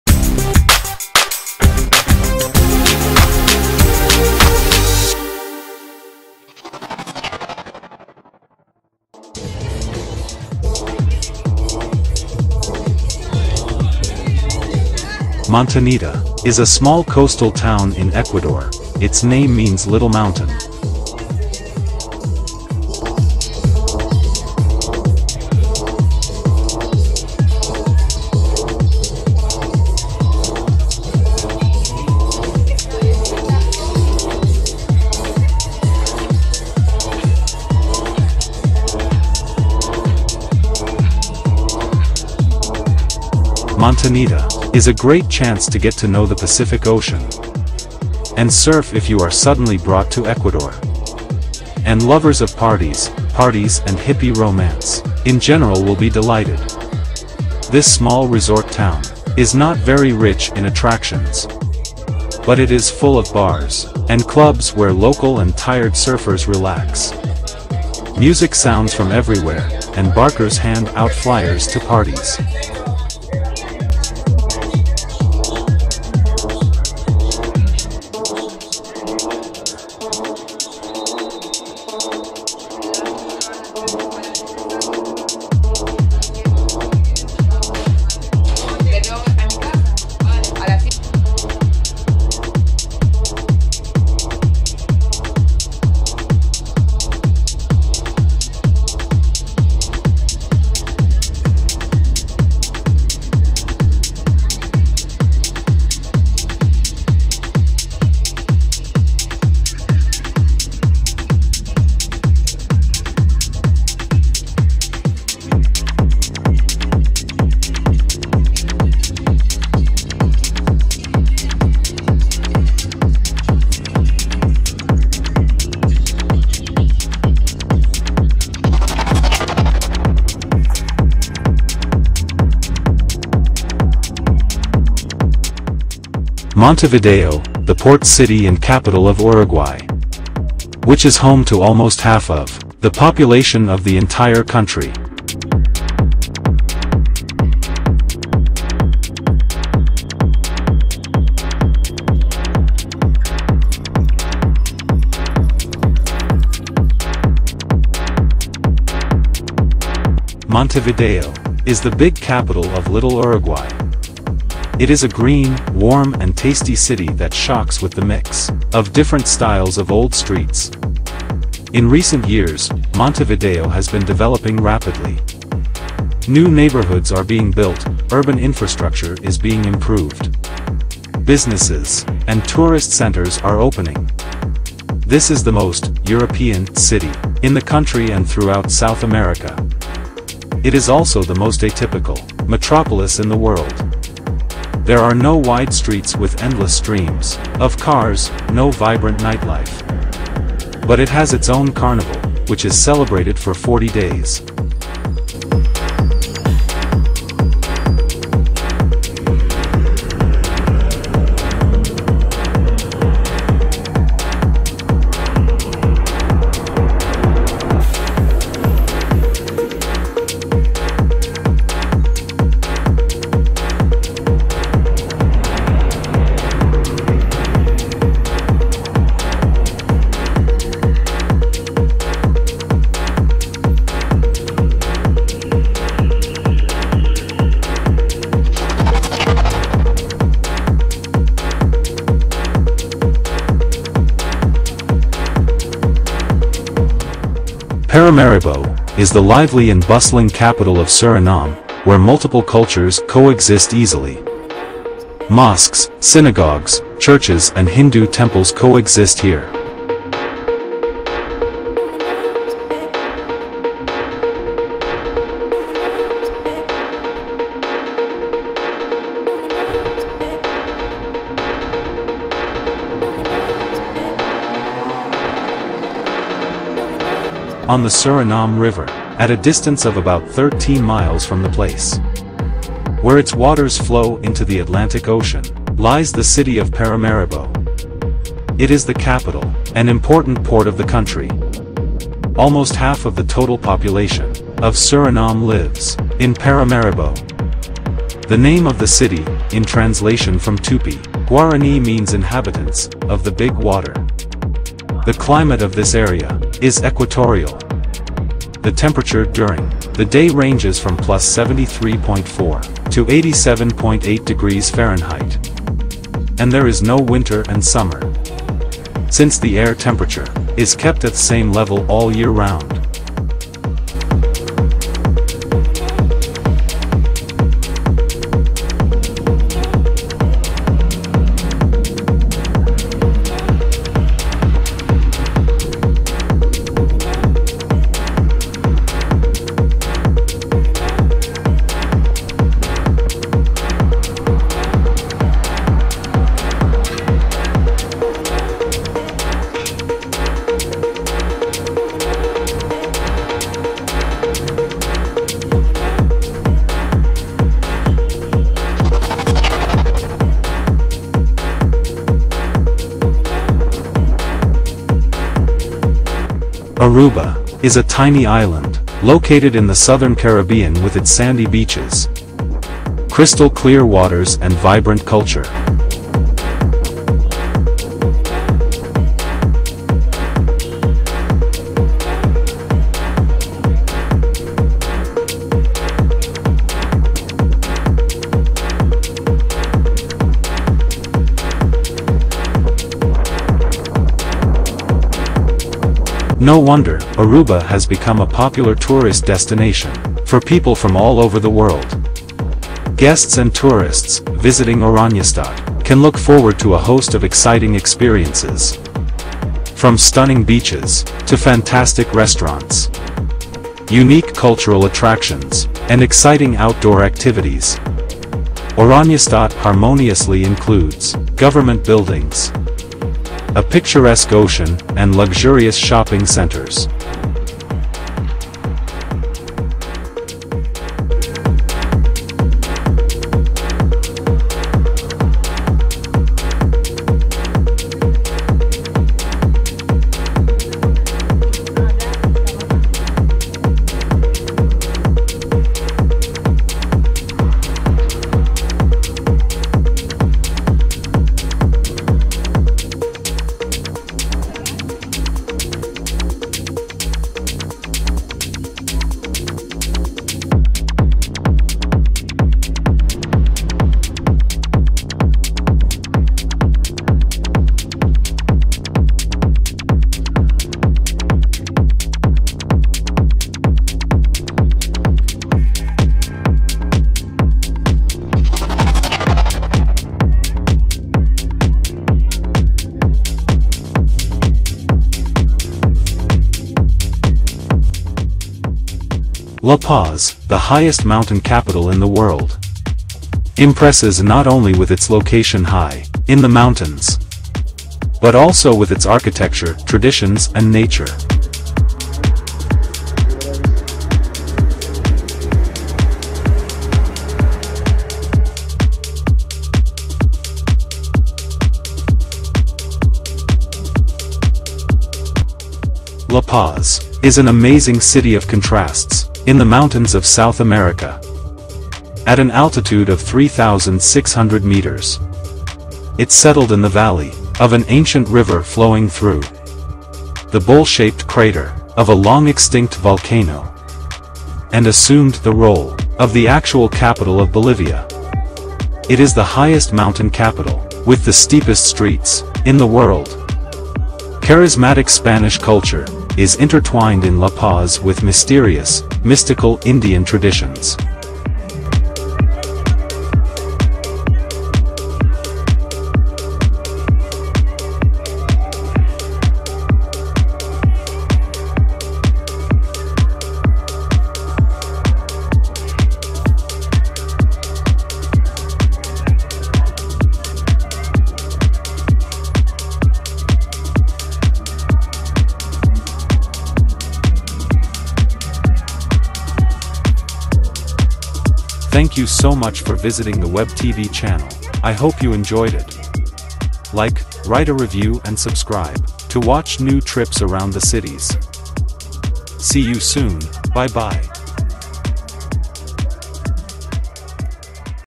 February Walk South America#travel
Hello! With you channel Web TV. We are together again, and it's great, are you ready for a walk, if so, then go ahead, walking around South America!
01. Day walk along Montanita. Ecuador
Montanita is a small coastal town in Ecuador, its name means "little mountain".
Montanita is a great chance to get to know the Pacific Ocean and surf if you are suddenly brought to Ecuador, and lovers of parties, parties and hippie romance in general will be delighted. This small resort town is not very rich in attractions, but it is full of bars and clubs where local and tired surfers relax, music sounds from everywhere, and barkers hand out flyers to parties.
02. Montevideo, walking tour. Uruguay
Montevideo, the port city and capital of Uruguay, which is home to almost half of the population of the entire country.
Montevideo is the big capital of little Uruguay. It is a green, warm and tasty city that shocks with the mix of different styles of old streets. In recent years, Montevideo has been developing rapidly - new neighborhoods are being built, urban infrastructure is being improved, businesses and tourist centers are opening. This is the most "European" city in the country and throughout South America. It is also the most atypical metropolis in the world - there are no wide streets with endless streams of cars, no vibrant nightlife, but it has its own carnival, which is celebrated for forty days.
03. Tour of Paramaribo. Suriname
Paramaribo is the lively and bustling capital of Suriname, where multiple cultures coexist easily. Mosques, synagogues, churches and Hindu temples coexist here.
On the Suriname River at a distance of about 13 miles from the place where its waters flow into the Atlantic Ocean lies the city of Paramaribo. It is the capital and important port of the country. Almost half of the total population of Suriname lives in Paramaribo. The name of the city in translation from Tupi-Guarani means "inhabitants of the big water". The climate of this area is equatorial. The temperature during the day ranges from +73.4 to 87.8 °F (+23 to +31 °С), and there is no winter and summer, since the air temperature is kept at the same level all year round.
04. Walking tour of Oranjestad. Aruba
Aruba is a tiny island located in the southern Caribbean with its sandy beaches, crystal clear waters and vibrant culture.
No wonder Aruba has become a popular tourist destination for people from all over the world. Guests and tourists visiting Oranjestad can look forward to a host of exciting experiences, from stunning beaches to fantastic restaurants, unique cultural attractions and exciting outdoor activities.
Oranjestad harmoniously includes government buildings, a picturesque ocean, and luxurious shopping centers.
05. Miraflores. La Paz. Bolivia
La Paz, the highest mountain capital in the world, impresses not only with its location high in the mountains, but also with its architecture, traditions and nature.
La Paz is an amazing city of contrasts in the mountains of South America, at an altitude of 3600 meters. It settled in the valley of an ancient river flowing through the bowl-shaped crater of a long-extinct volcano, and assumed the role of the actual capital of Bolivia. It is the highest mountain capital with the steepest streets in the world. Charismatic Spanish culture is intertwined in La Paz with mysterious, mystical Indian traditions.